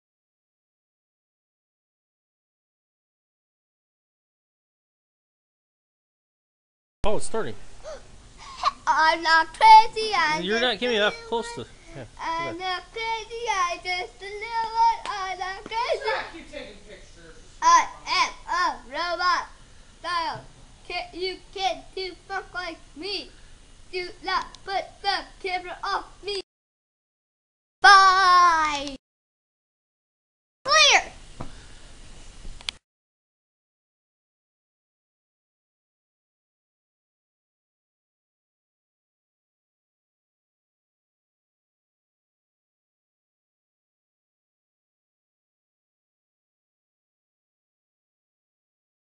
the jacob show ep 1 un crazy robot!
it's my new tv show! yayh! introducing, my mom and new webkinz! cocker spaniel is buster and brown arabian is snowspot! ( because of his spot!) no mean comments! and no comments about the white spot! comment, video responds, and rate! and don't forget to stay tune for my next episode! this is my first time my dad recorded me on his new camera for video!